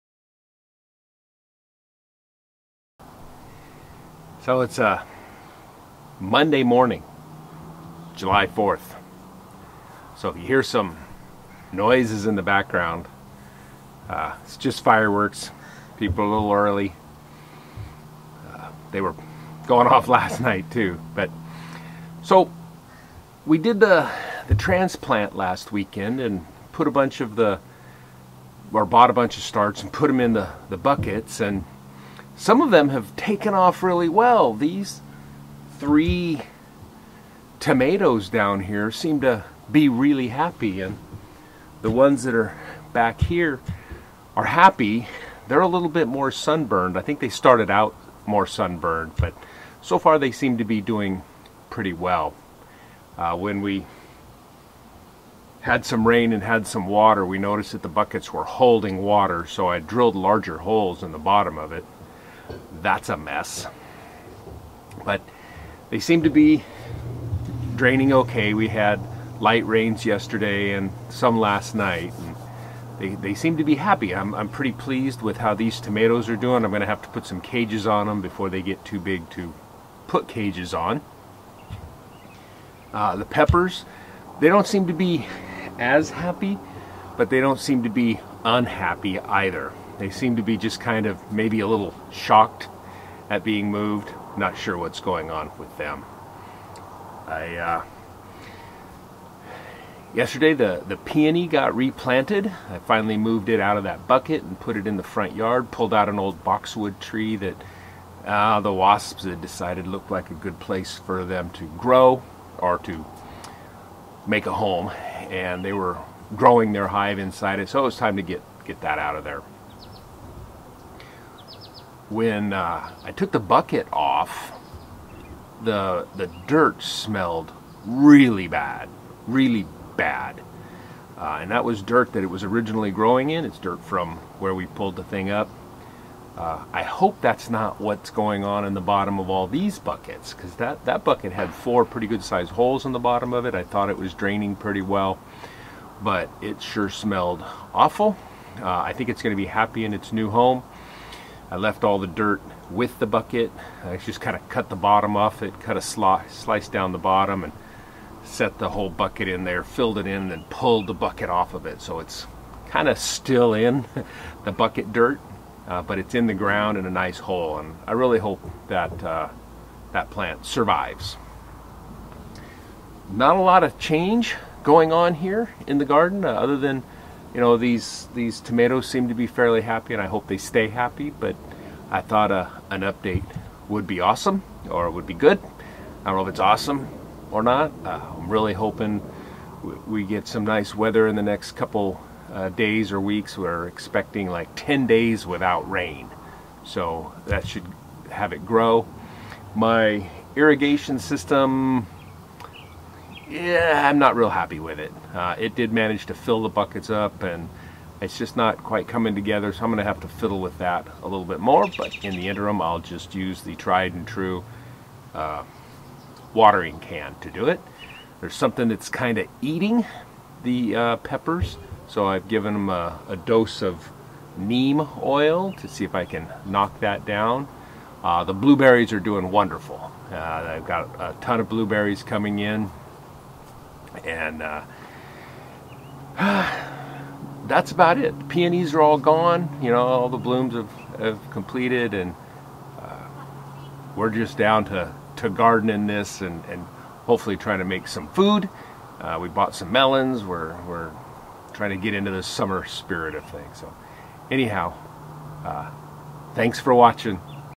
S3:E6 - Bucket Garden 2022 - It's not dead yet!!!
Update on how it's going and how it's not dead!!